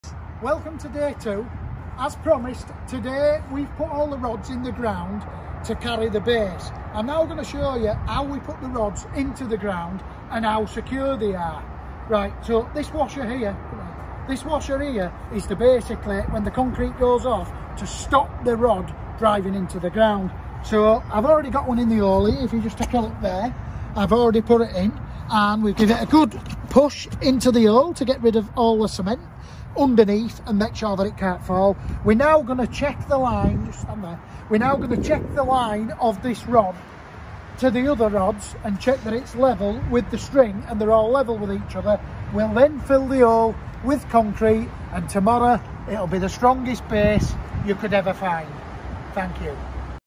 Day 2 of a garden office construction with full tutorial
Day 2 and the threaded bar is concreted into the pre dug pile holes. All the rods are in line using the string lines we set out the day before. This ensures that are perfectly in line and fit into the drilled frame timbers we will fit on day 3. We mix the concrete on site at a ratio of 4:1, 4 parts ballast and 1 part cement. A messy day as the site is muddy and mixing the concrete can be a messy job.